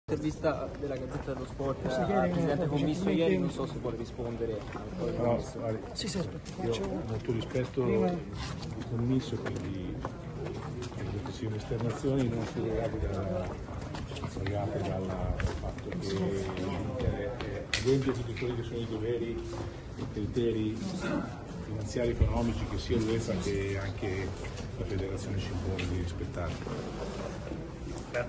INTER-FIORENTINA, SCONTRO DIALETTICO: LA RISPOSTA DI MAROTTA A COMMISSO
La risposta di Beppe Marotta alle parole di Commisso